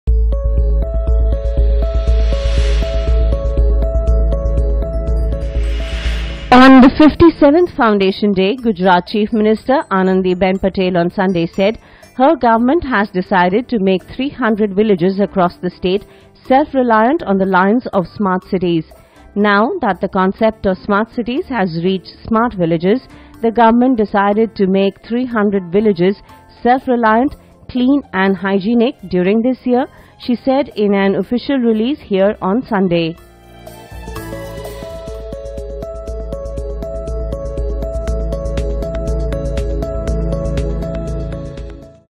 300 Smart Villages To Be Developed In Gujarat : CM Anandiben Patel
On 57th Foundation Day, Gujarat chief minister Anandiben Patel on Sunday said her government has decided to make 300 villages across the state self-reliant on the lines of smart cities.